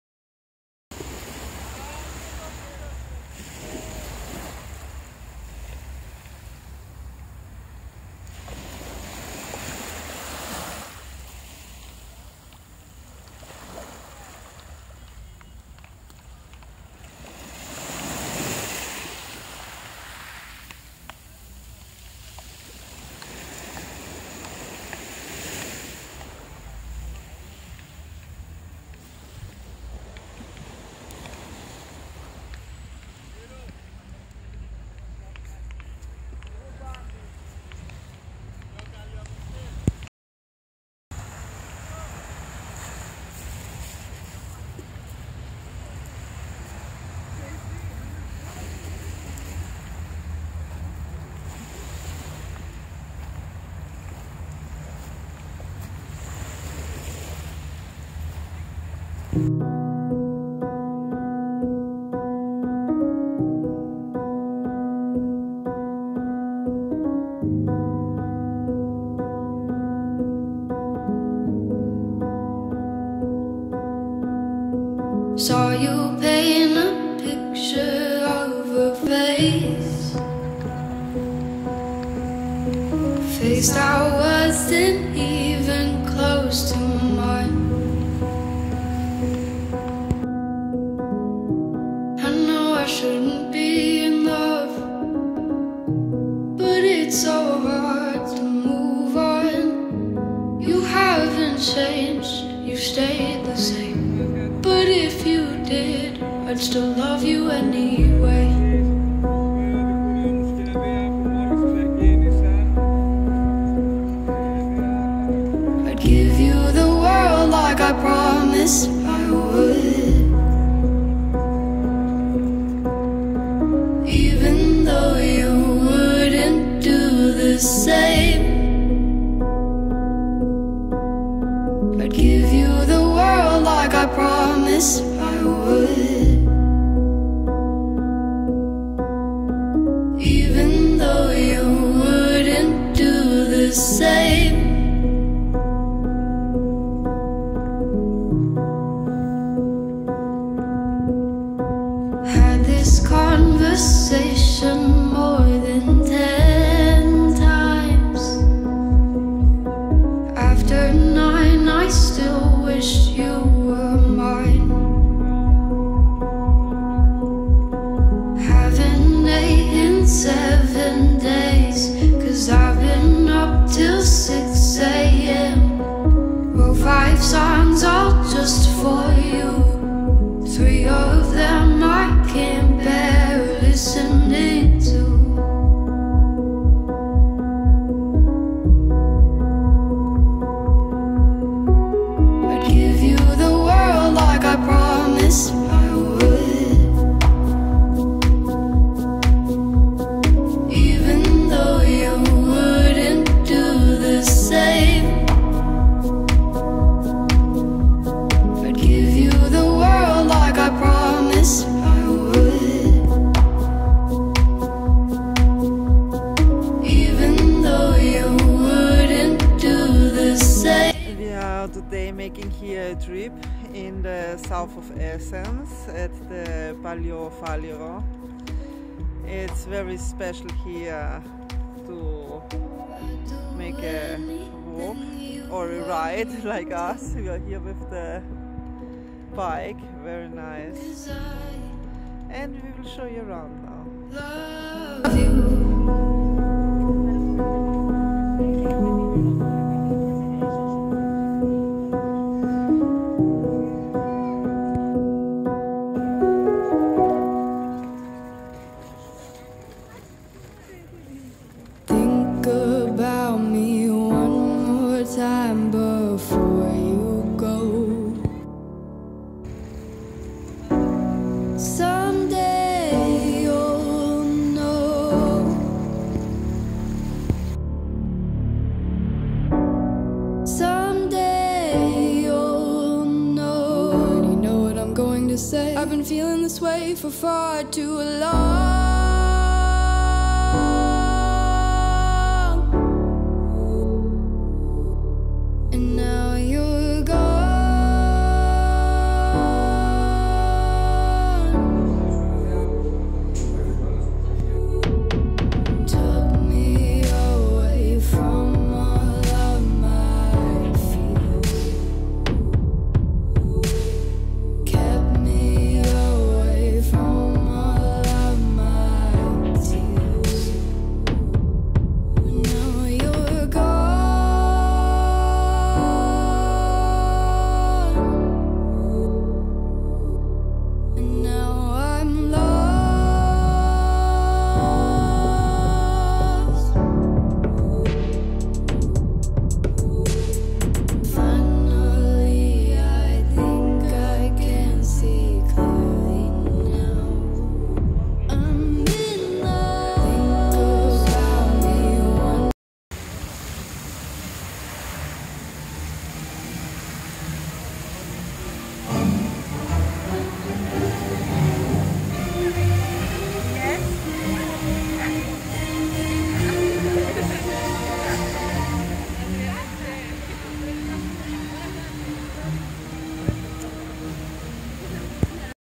Athens in Greece vlog 2022 🇬🇷
A trip to the South of Athens ,Greece 2022  .

unpaid Advertising
00:00   intro

01:45 Niarchos Foundation

02:41 Palaio Faliro

05:06  The Shusi Bar

05:55  Niarchos Foundation

I want to introduce you to my Ringana Shop -Business